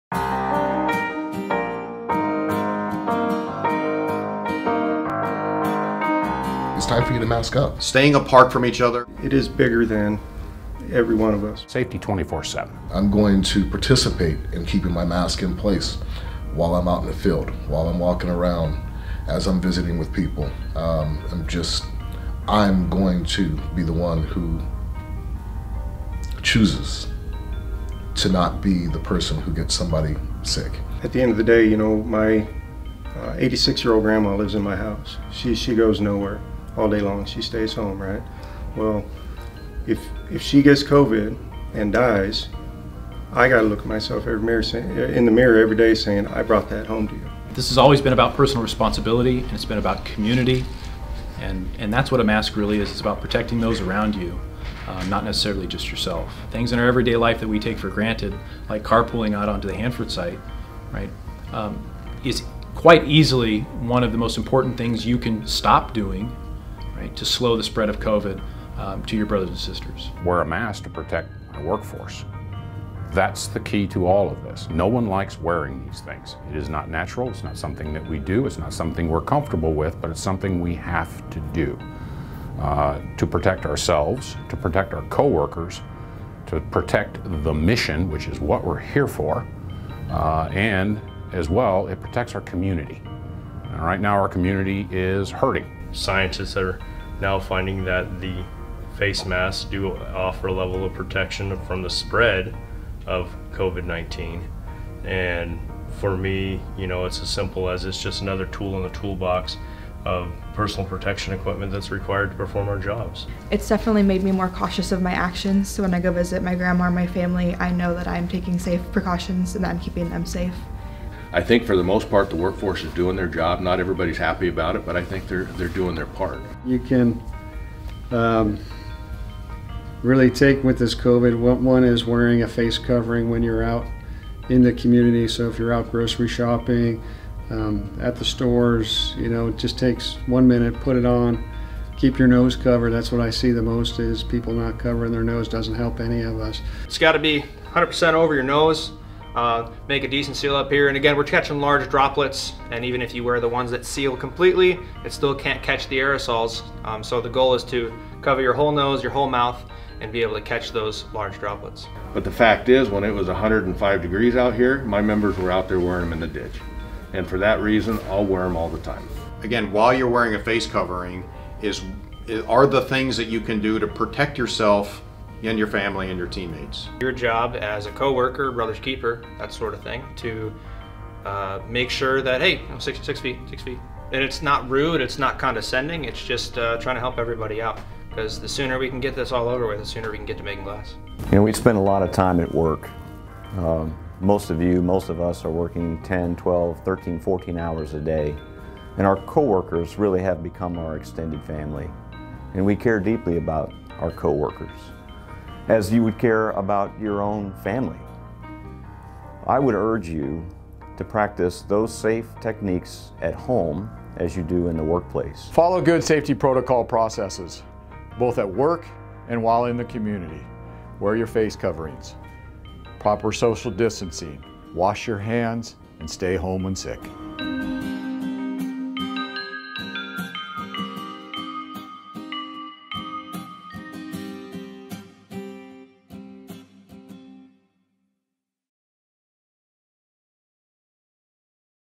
HANFORD STOP THE SPREAD
Hanford leadership talks about the importance of following COVID-19 safety protocols.